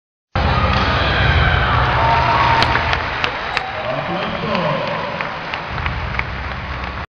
Open Pal-Li 2015 Rompimiento
Open Pal-Li 2015 disputado en la ciudad de Barquisimeto Venezuela, mejorando cada ves mas nuestro Taekwondo